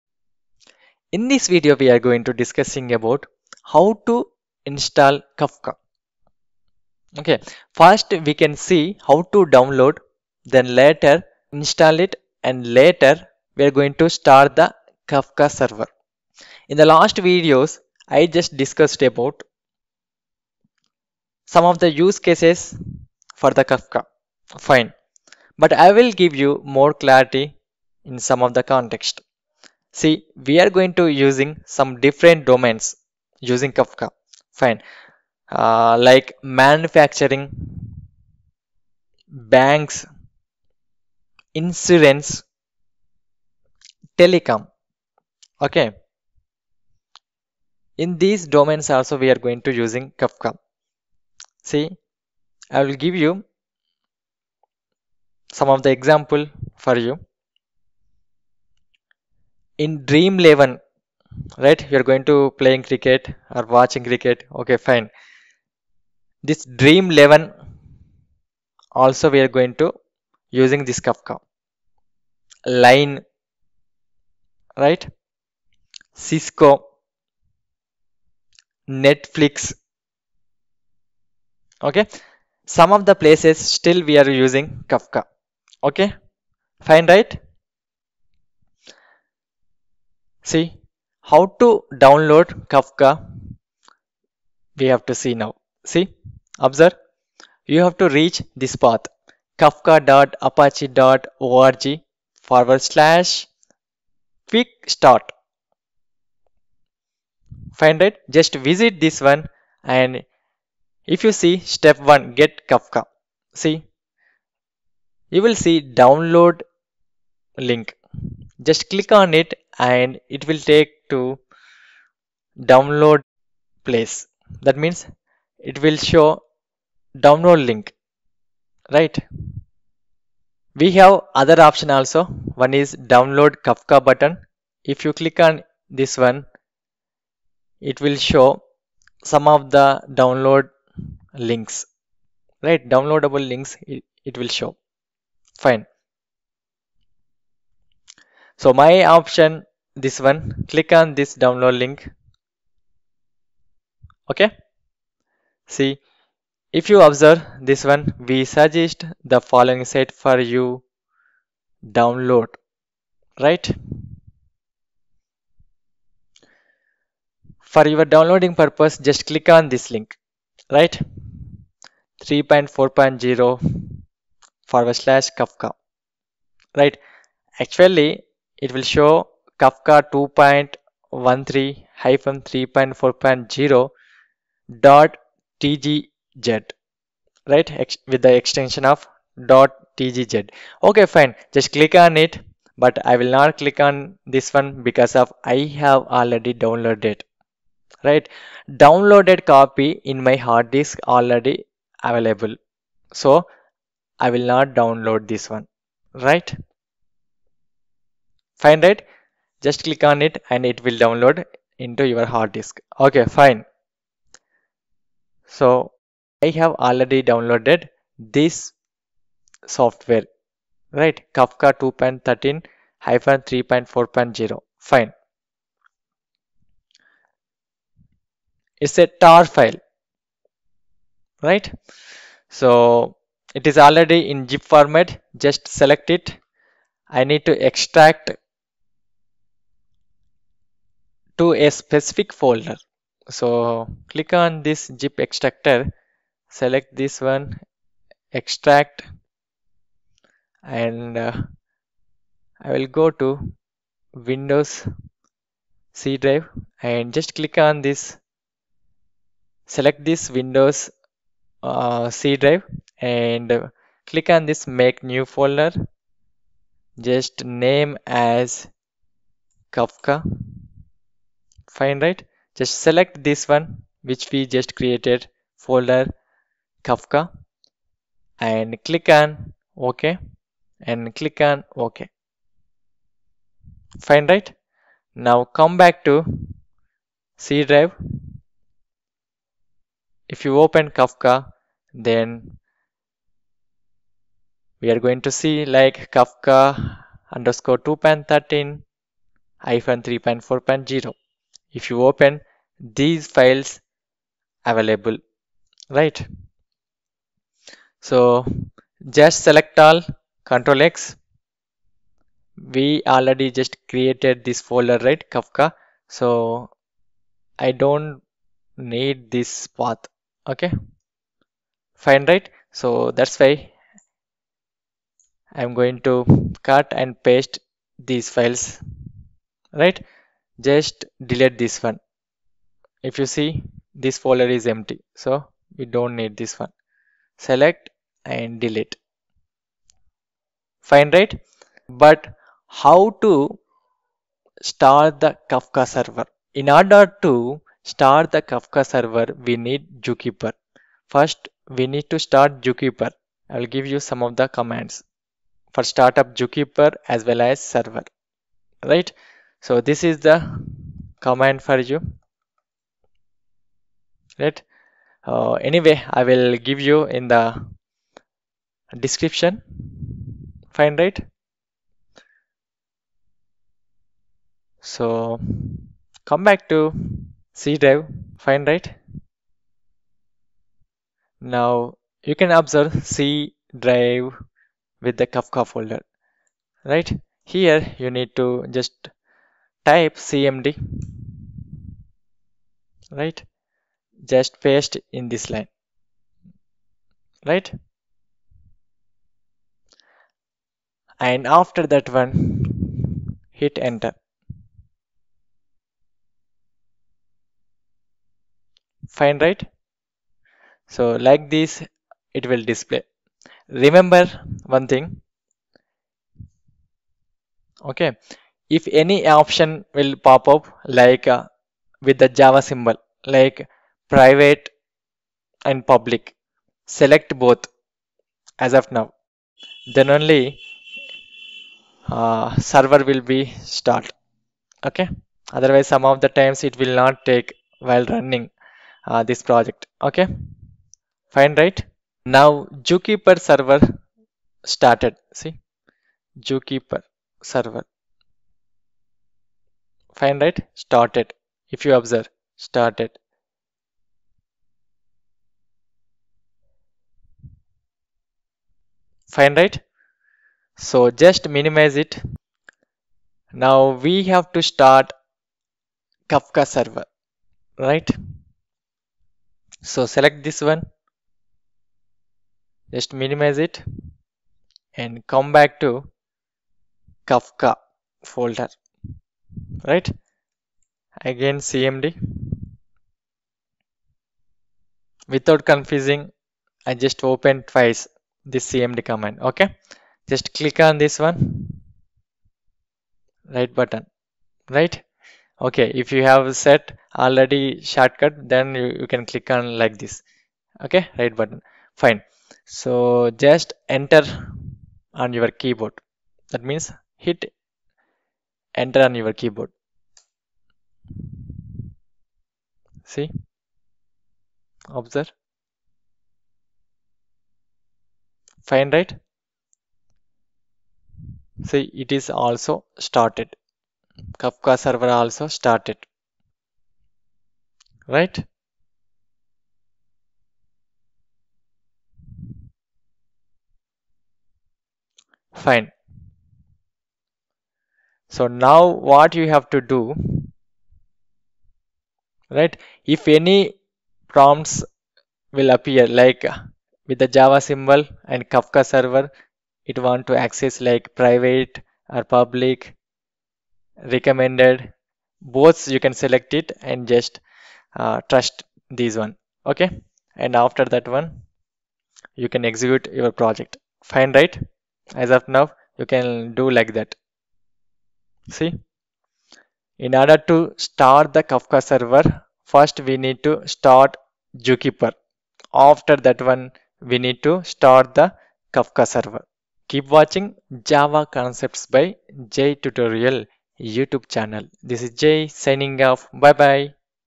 Apache Kafka download, Installation and Start up the Zookeeper and Kafka Server by Ponnam Jay
In this video; I explained about Apache Kafka download, Installation and Start up the Zookeeper and Kafka Server
Commands for Start Zookeeper and Kafka Server:::

ZooKeeper:::

.\bin\windows\zookeeper-server-start.bat .\config\zookeeper.properties

Kafka Server:::

.\bin\windows\kafka-server-start.bat .\config\server.properties